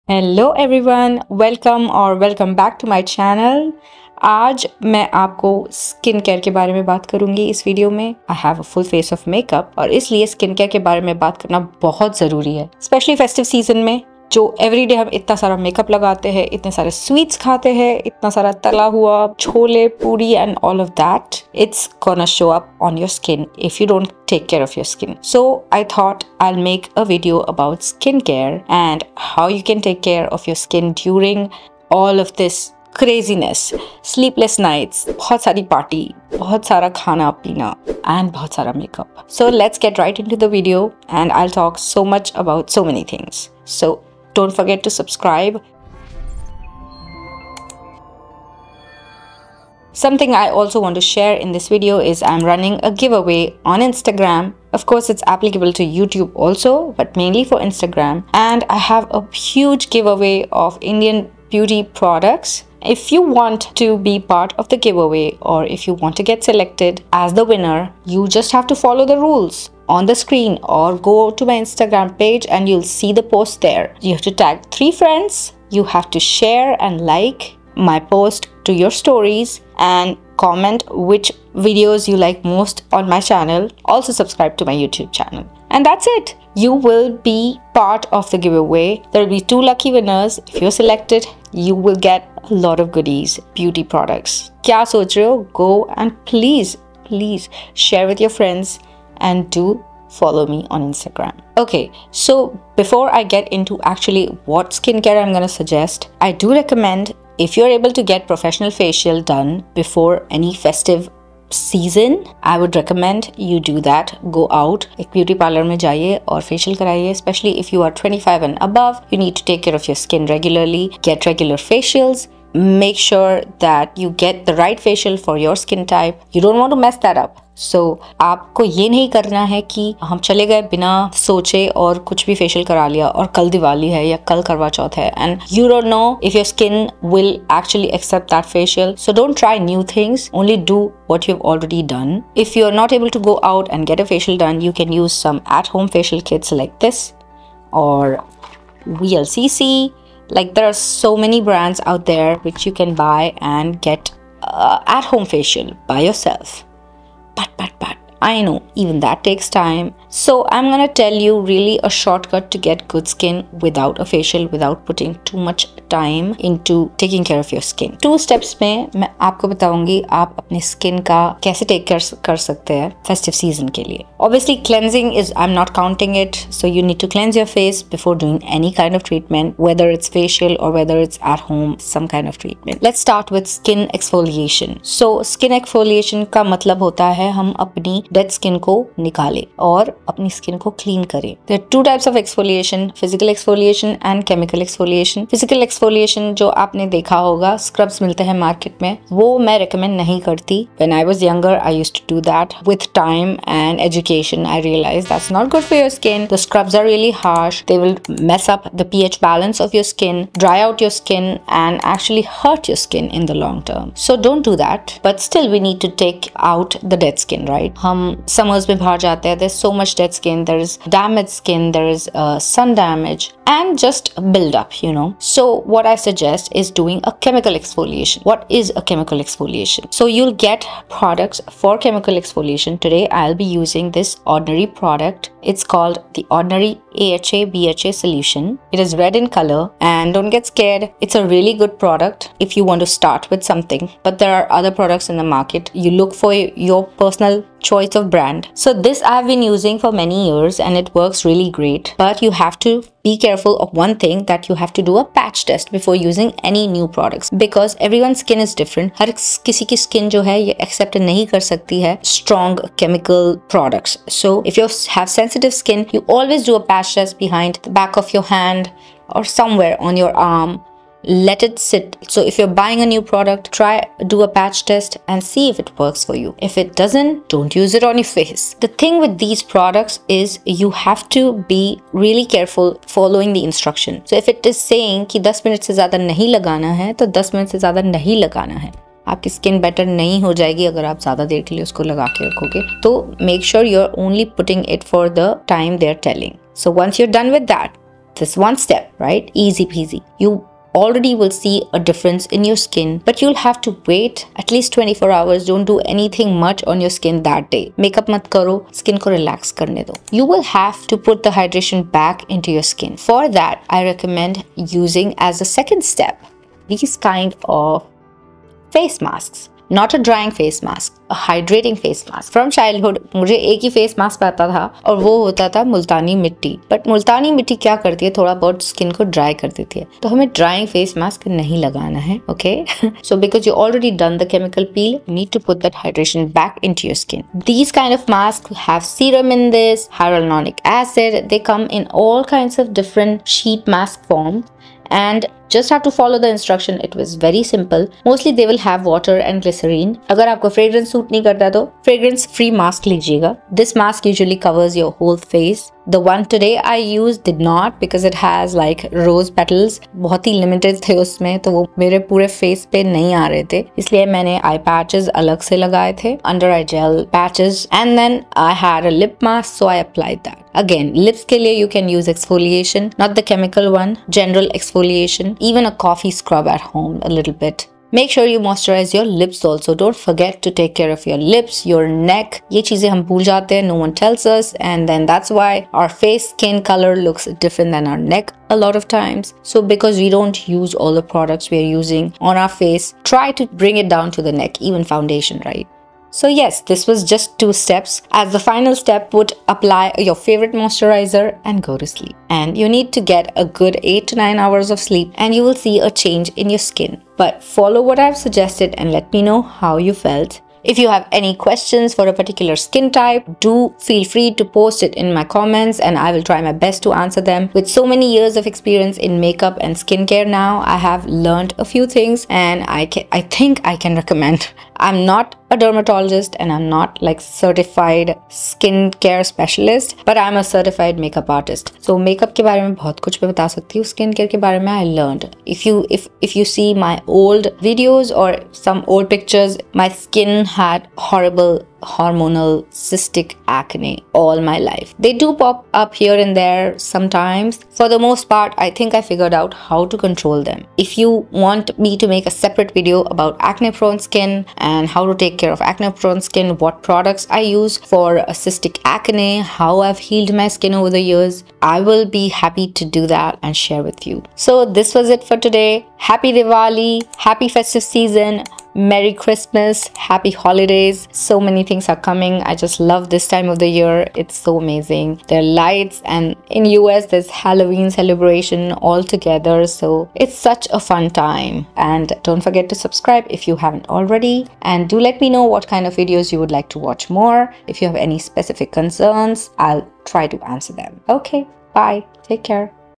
2 Step Skincare from dull to glowy skin at Home | Post Festive Skincare | At home Facial
Looking for a simple yet effective skincare routine? In this video, I’m sharing a 2-step skincare routine that combines the power of chemical exfoliation with the soothing benefits of a hydrating face mask.

Discover how to gently exfoliate your skin to remove dead cells and impurities, followed by a nourishing mask to replenish moisture and leave your skin glowing and refreshed.

Whether you’re tackling dullness, uneven texture, or just want to give your skin some extra love, this routine is perfect for all skin types (with a few tips to customize it!).

✨ Products Used:
 • The Ordinary AHA/BHA Peeling Solution
 • Hydrating face mask

💡 Pro Tips:
 • Always patch test before using new products.
 • Don’t forget sunscreen during the day to protect your freshly exfoliated skin!

♡ ♡ Ekta Bhardwaj ♡ ♡